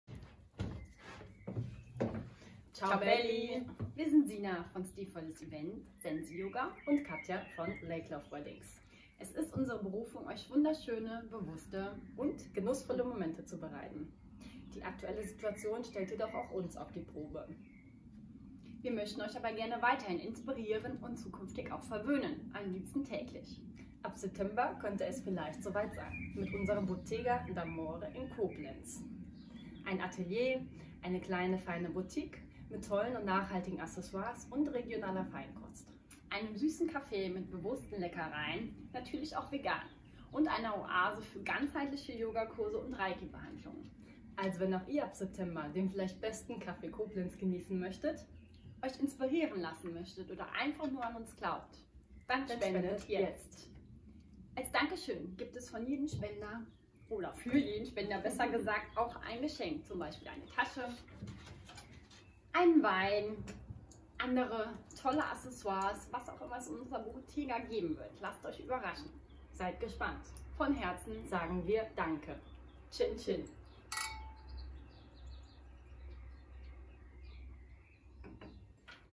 Teaser Bottega d'Amore - Von Herzen
Bottega d'Amore - Von Herzen ist unser Traumprojekt, welches wir ab September für euch in Koblenz erlebbar machen möchten. Was ist die Bottega d'Amore? Atelier - Boutique - Café - Events - Yoga - Reiki
Unterstützt uns jetzt und lasst euch ab September täglich von uns inspirieren und verwöhnen. Wir freuen uns auf euch und sagen von Herzen danke für eure Unterstützung!
Cin Cin,
eure Zwillinge.
Sina und Katja